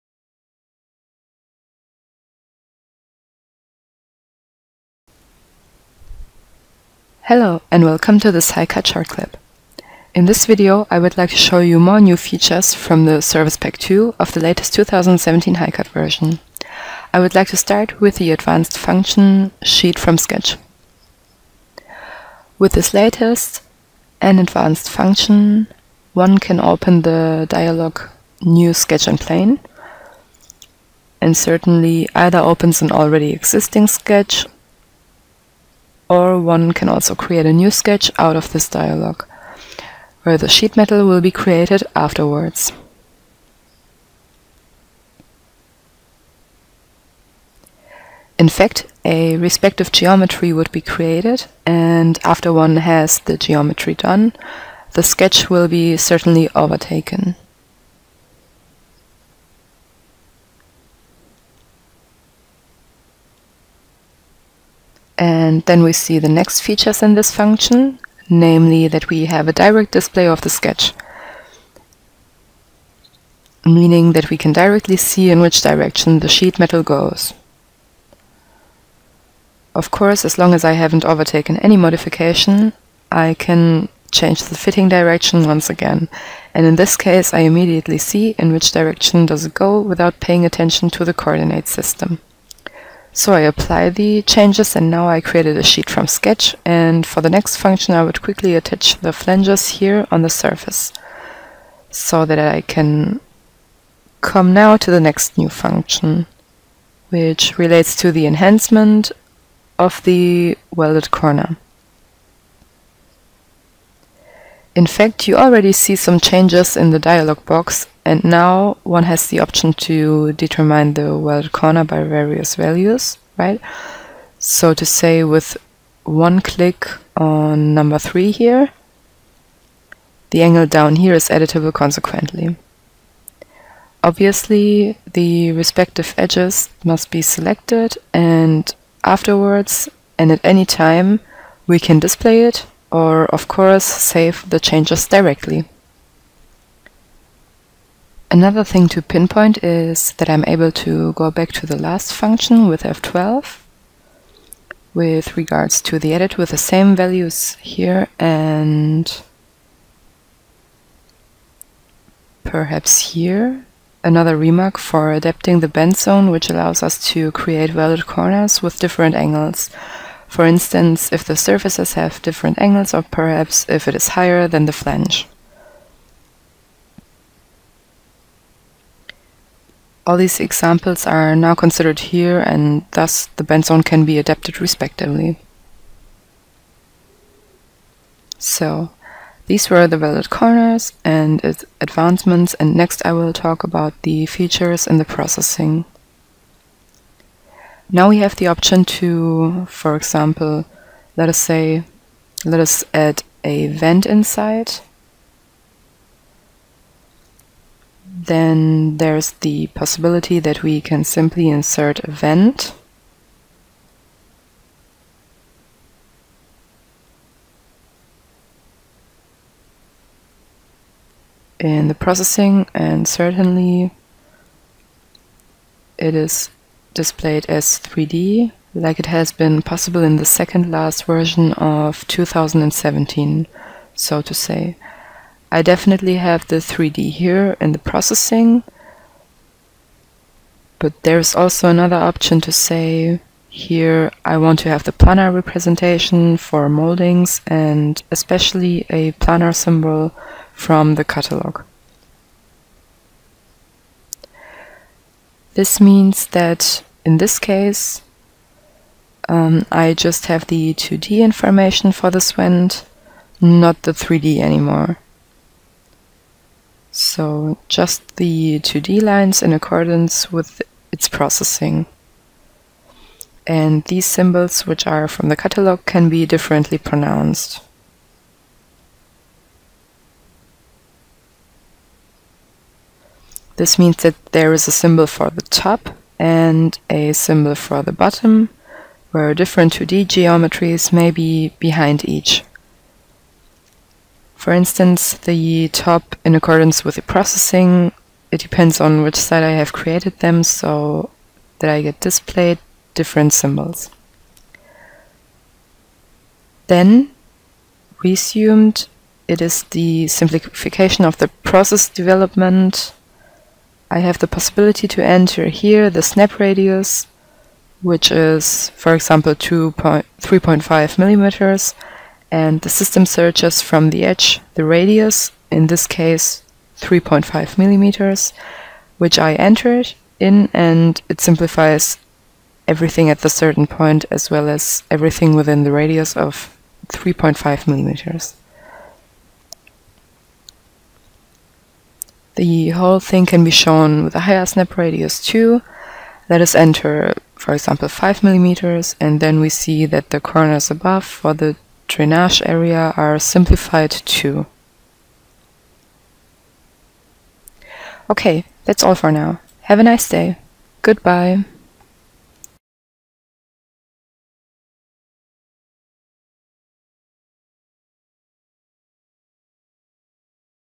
HiCAD 2017 SP2 | ShortClips: Sheet Metal News
In our ShortClip about new functions in the Sheet Metal module we will show you the enhanced 3-D Sheet development function and the revised “Sheet from sketch” and “Welded corner” functions.